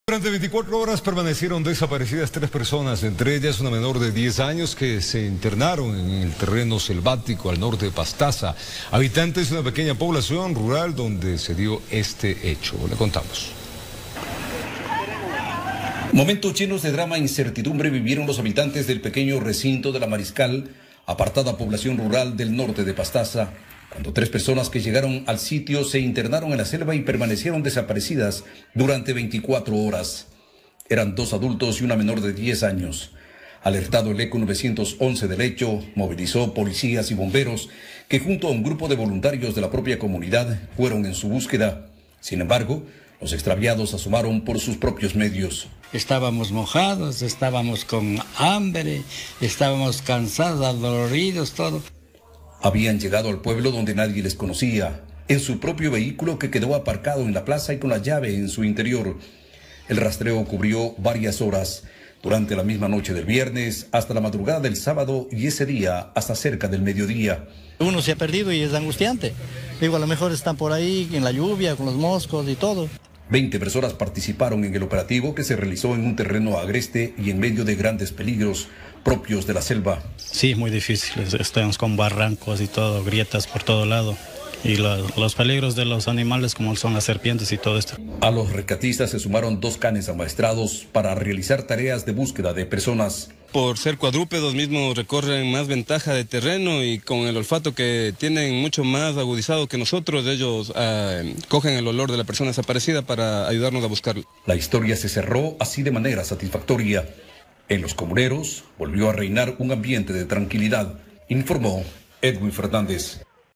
Rescatan a tres personas que estuvieron desaparecidas por más de 24 horas
Durante más de 24 horas, tres personas permanecieron desaparecidas, entre ellas una menor de 10 años, luego de internarse en una zona selvática de Pastaza.